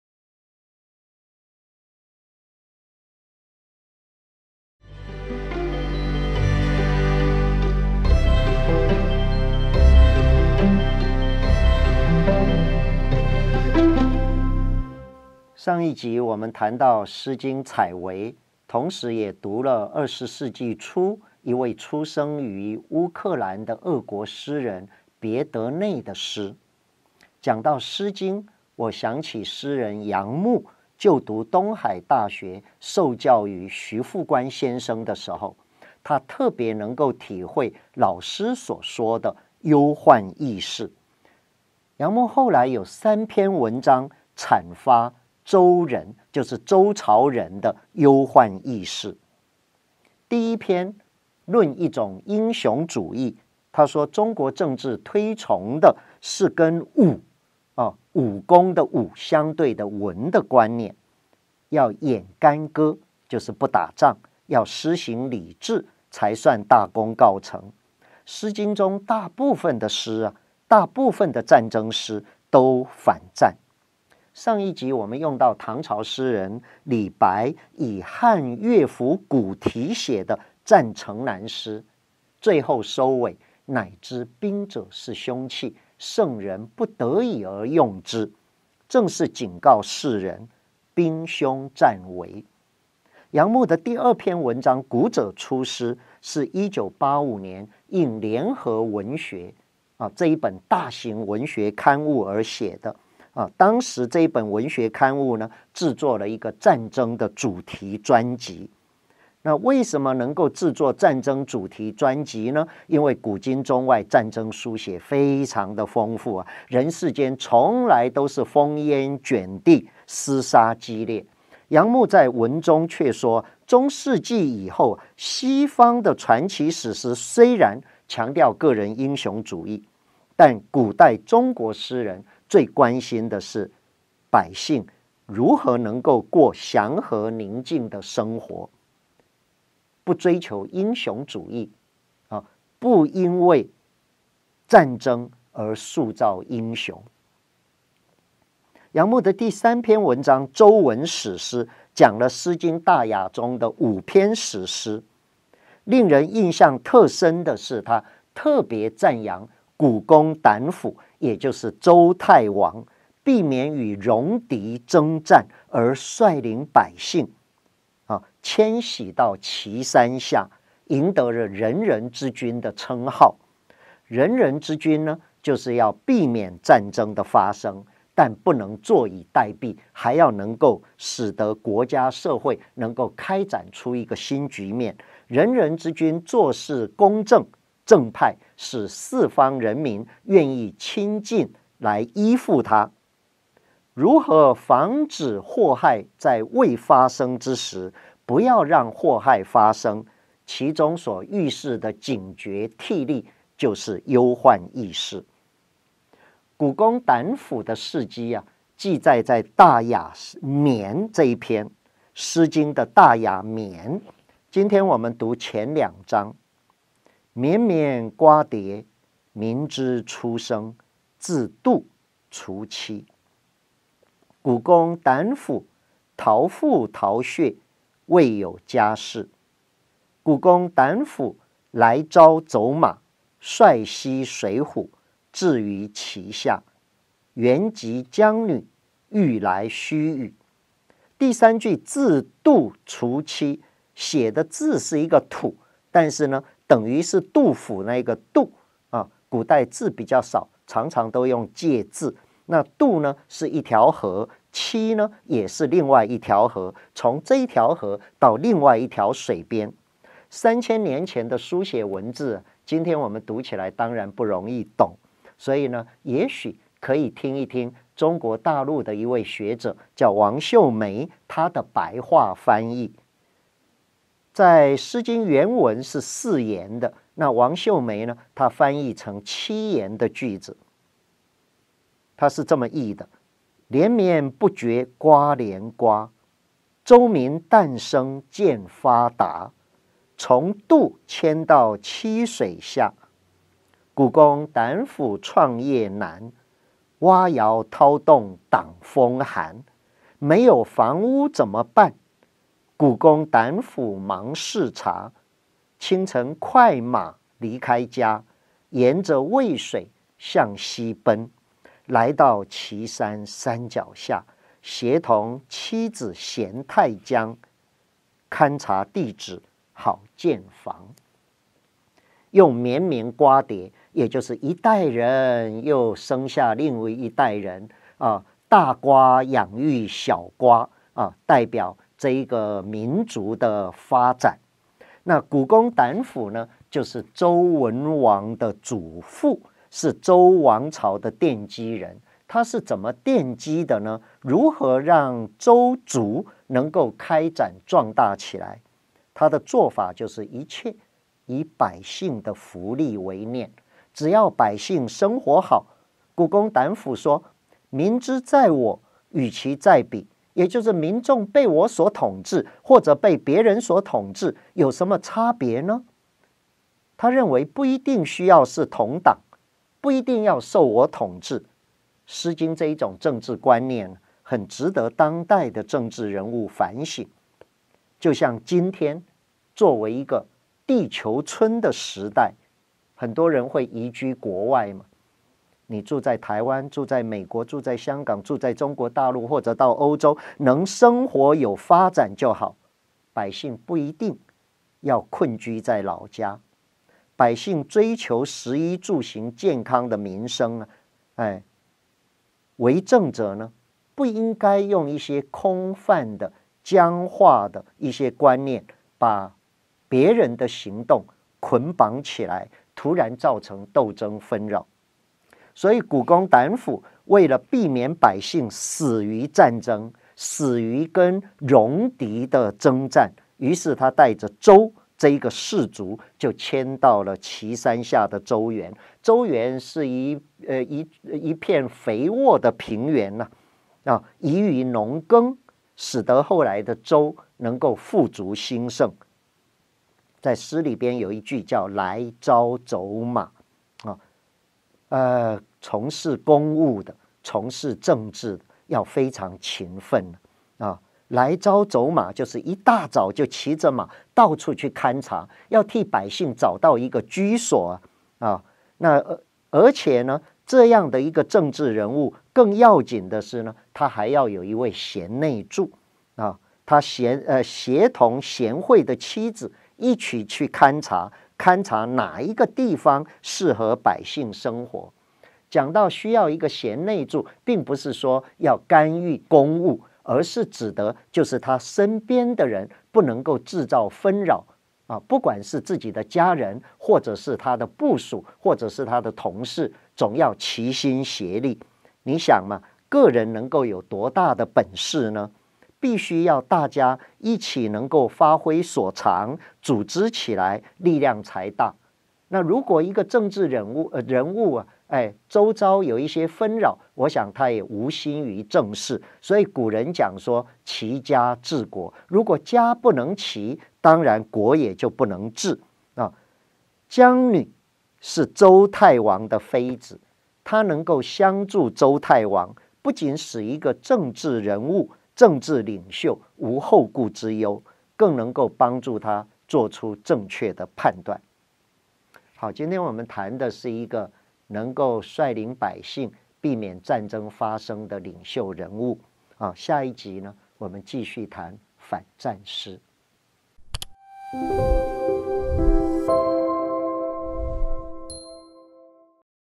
🖋️ 遇見一首詩-反戰詩02
【遇見一首詩】反戰主題來到第二集，義芝老師藉由詩人楊牧所寫的三篇作品，由詩經看當時的政治觀，這集非常適合當代政治人物一讀，尤其談到賢內助如何扮演好輔助的角色，都蠻值得最近選舉風雲再起的政治人借鏡思考。

💡更多詩靈感就在【遇見一首詩】節目：趨勢影音 || Youtube頻道